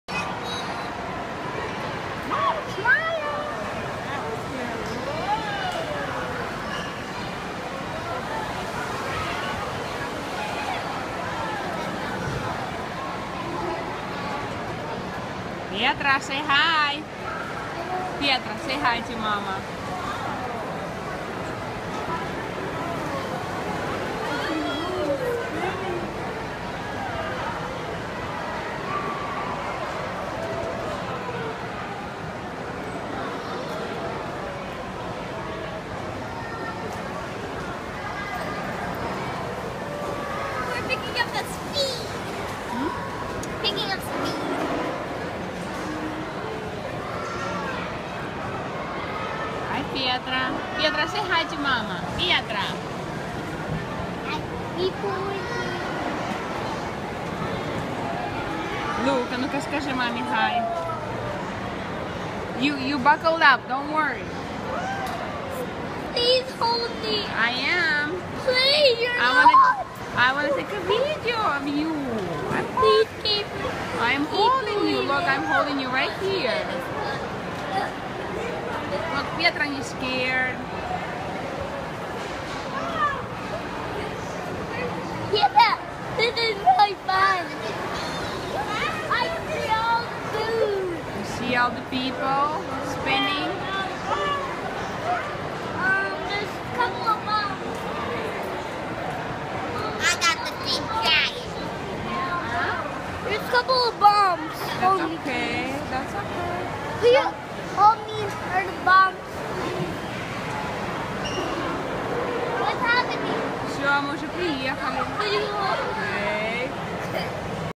Bella 9 yr. Luke 5 yr. and petra 3.10 yr
Катаются на Лошадках